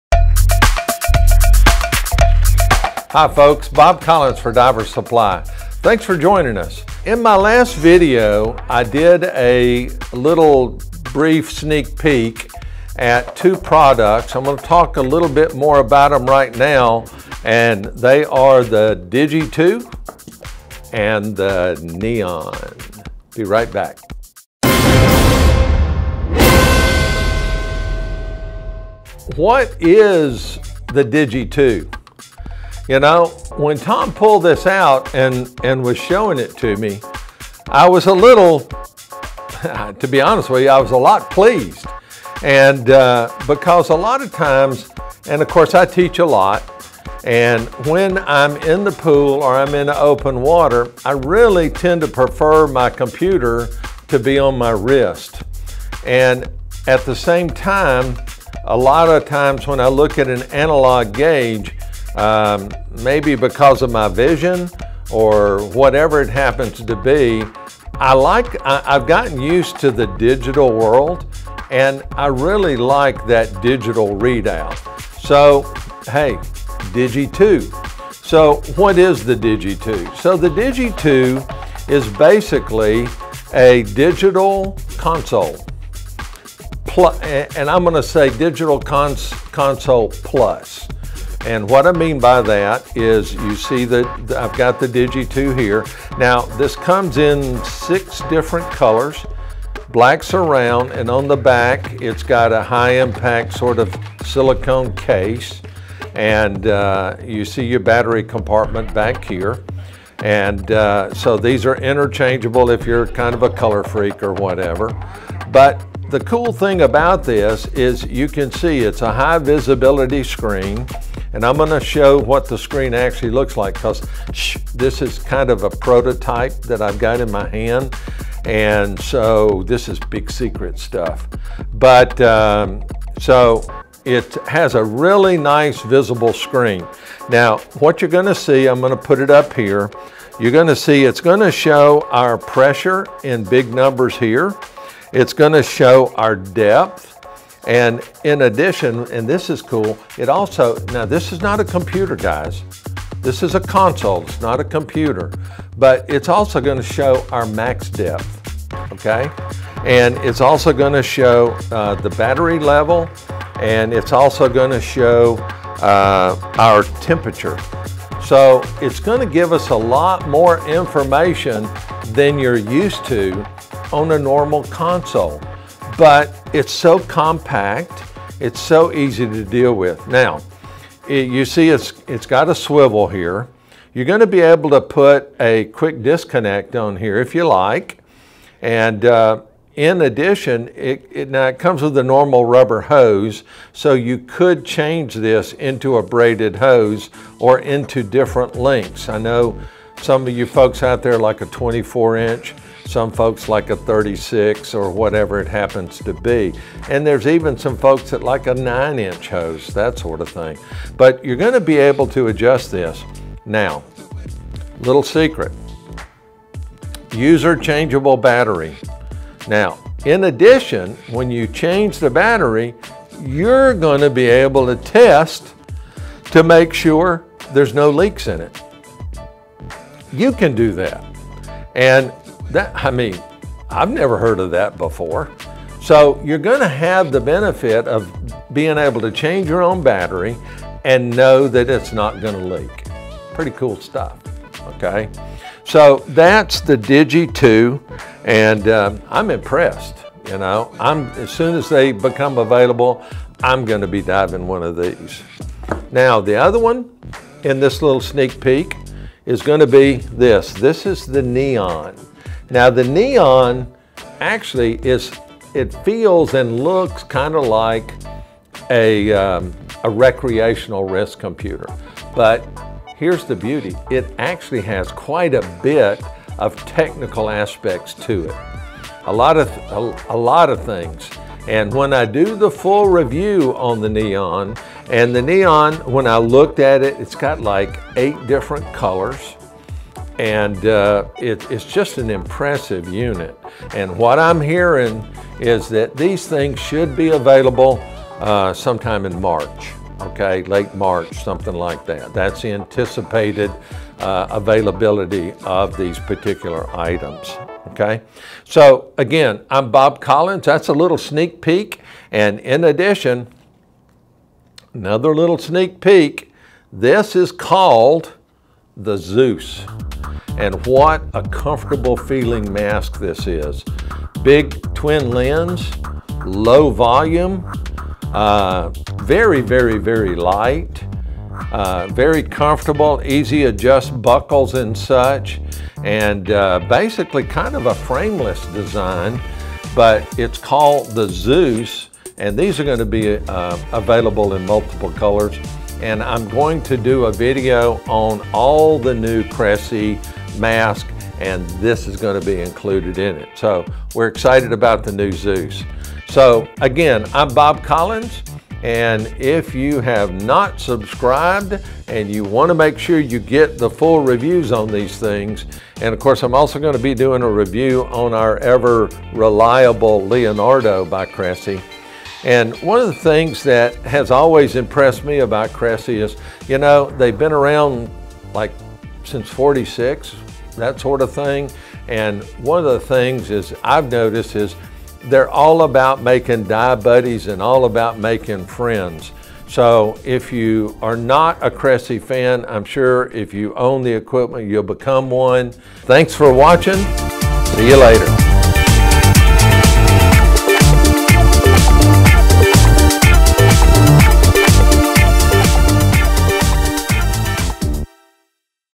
Cressi DIGI 2 and NEON sneak peek *** Digital Dive Console
The Cressi DIGI 2 and NEON are new releases for 2019.  DIGI 2 is a Digital Dive Console and NEON is a personal wrist computer.  Find:

The Digi 2 is a new concept in pressure gauge systems by Cressi.  It mixes some features of a dive computer into a pressure/depth console.
This sneak peek looks at a few of the features and a full in water review will be along soon.  Look for this unit late March 2019.

The Cressi Neon is a new watch style personal dive computer that is feature rich and very affordable.  Also coming in March 2019